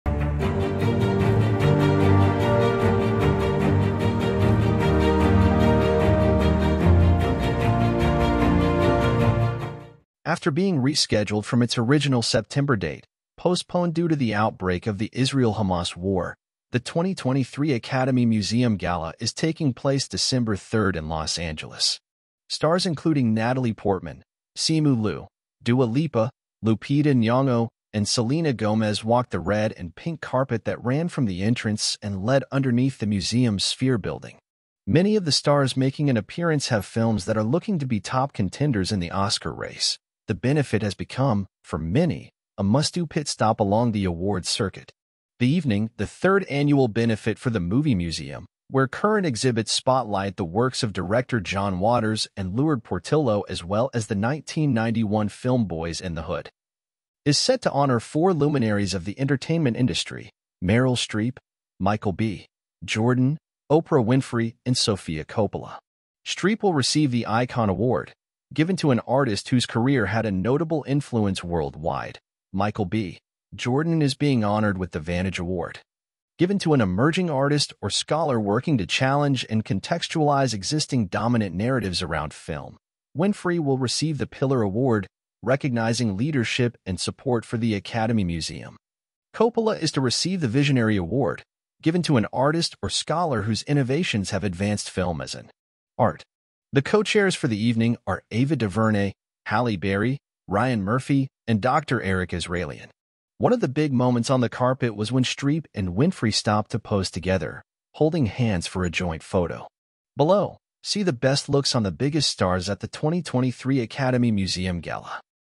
Oprah Winfrey Meryl Streep Selena Gomez and More Stars Walk the Red Carpet at the Acadmy Museum Gala
After being rescheduled from its original September date — postponed due to the outbreak of the Israel-Hamas War — the 2023 Academy Museum Gala is taking place Dec. 3 in Los Angeles. Stars including Natalie Portman, Simu Liu, Dua Lipa, Lupita Nyong’o and Selena Gomez walked the red and pink carpet that ran from the entrance and led underneath the museum’s Sphere building. Many of the stars making an appearance have films that are looking to be top contenders in the Oscar race; the benefit has become, for many, a must-do pit stop along the awards circuit.

The evening — the third annual benefit for the movie museum, where current exhibits spotlight the works of director John Waters and Lourdes Portillo as well as the 1991 film Boyz n the Hood — is set to honor four luminaries of the entertainment industry: Meryl Streep, Michael B. Jordan, Oprah Winfrey and Sofia Coppola.